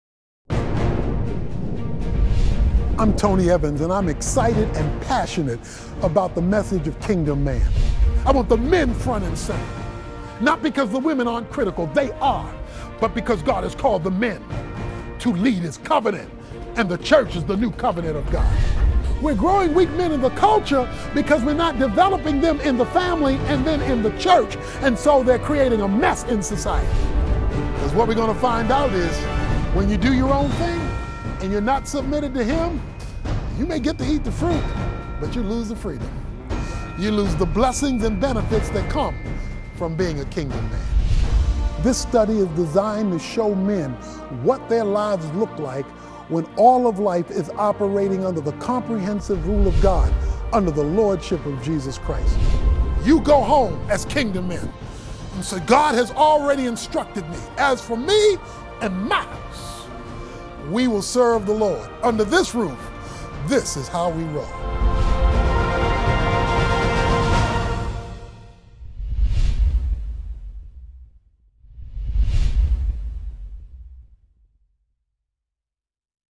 Kingdom Man video trailer
Kingdom Man is a 6-session Bible study for men by Dr. Tony Evans. This study is designed to empower men to exercise the God-given dominion they were created for by giving them a Kingdom understanding, vision, and perspective on their identity and responsibilities along with practical steps on how to become a man who rules his world well.

Our culture has redefined manhood by emasculating men through a repositioning of the role. Men are portrayed as weak, self-serving, and unfocused. The Bible clearly communicates that man was created to exercise dominion over the various areas of his life, taking responsibility for himself and others entrusted to his care. When a man functions within the principles of biblical manhood, those around him benefit from his leadership and care. This study challenges and equips men to fully understand their position under God, learning to operate under the authority of Jesus Christ.